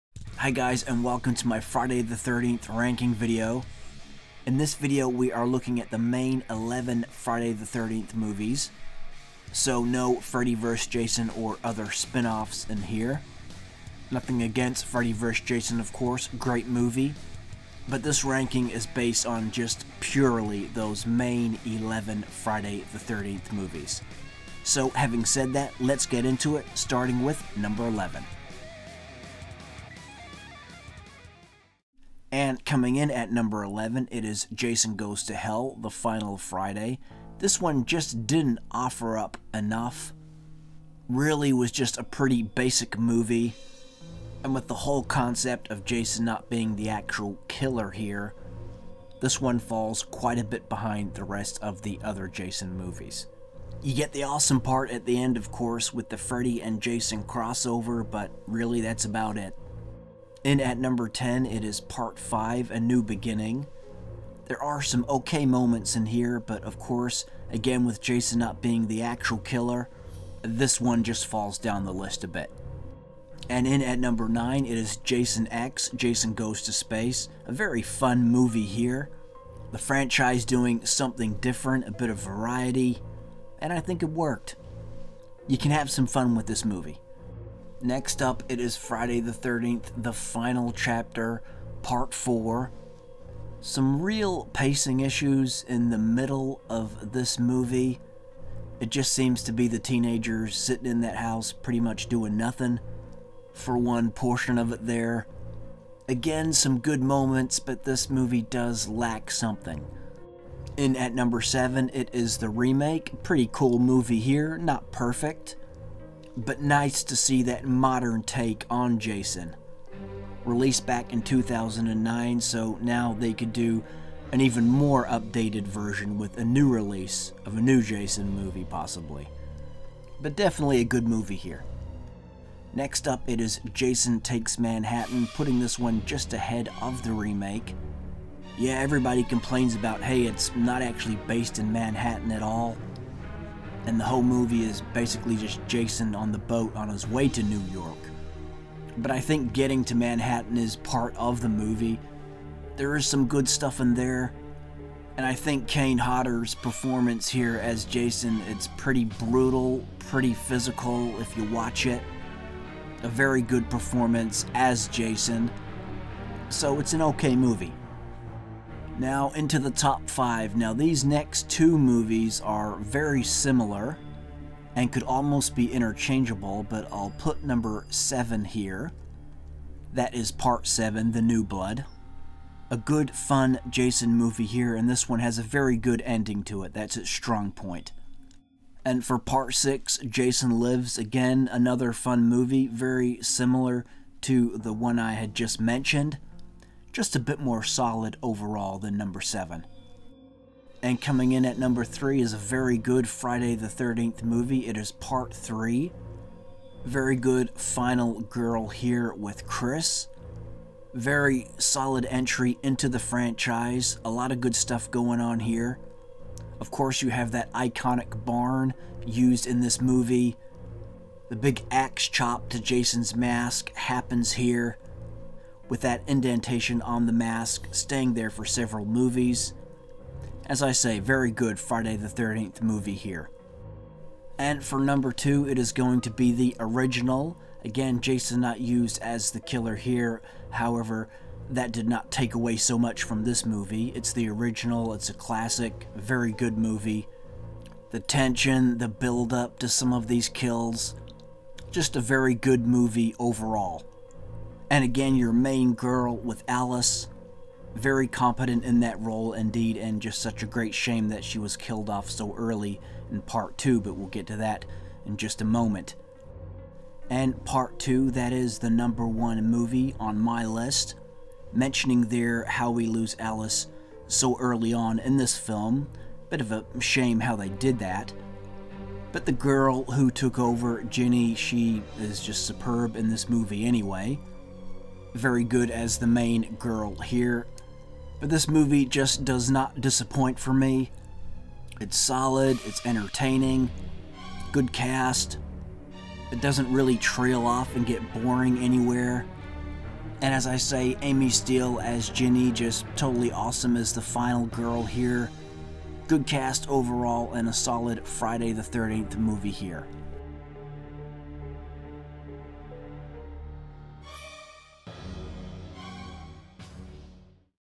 RANKING The 11 Friday 13th Movies
In this video i rank the main 11 Friday 13th movies that have been made to date & give my thoughts & opinions on each movie.

Consider hitting that SUPER THANKS button!

Footage courtesy Paramount Pictures